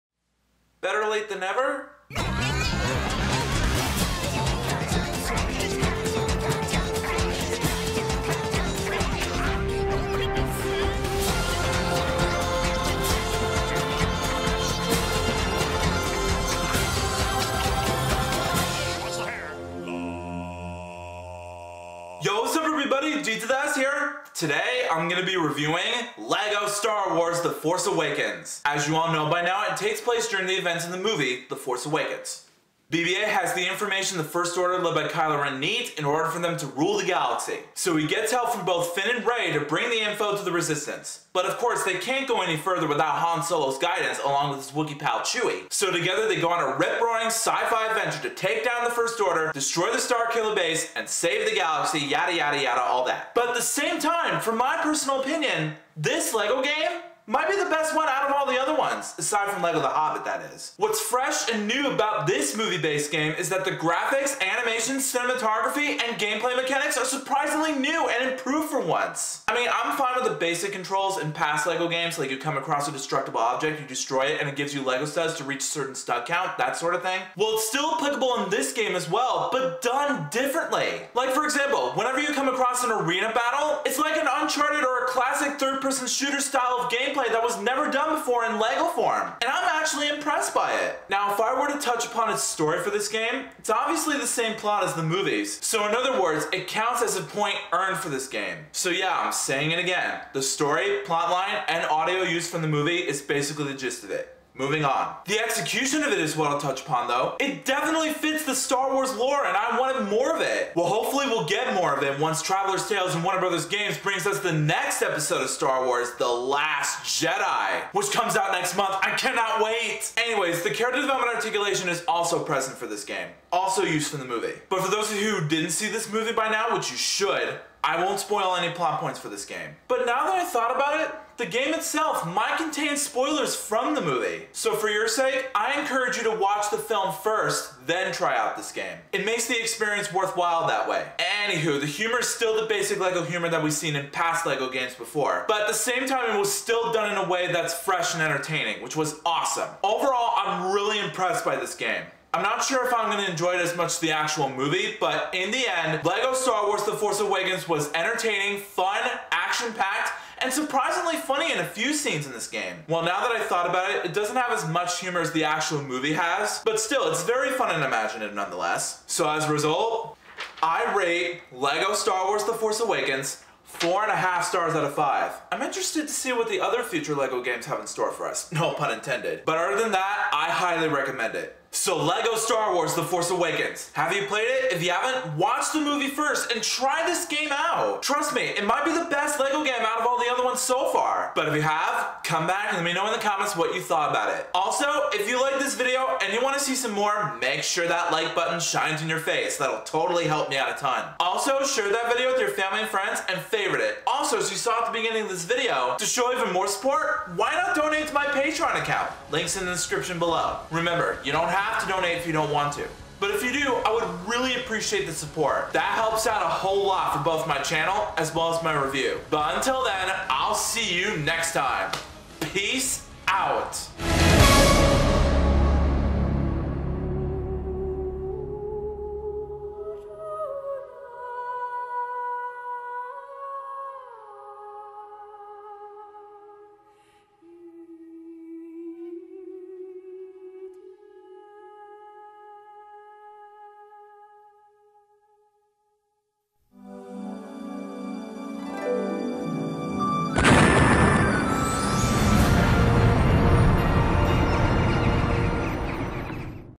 "LEGO Star Wars: The Force Awakens" - Game Review
Based on the world-wide phenomenon of the same name, you have the option to play as one of your favorite characters as you reenact the movie, but with a LEGO twist. Join them as you beat up stormtroopers, take on Kylo Ren and build your way to save the galaxy, brick by brick in their latest LEGO adventure, "LEGO Star Wars: The Force Awakens". Now on PS4, PS3, PSVITA, XBOX ONE, XBOX 360, Wii U and Mac OS X for you to own and cherish forever.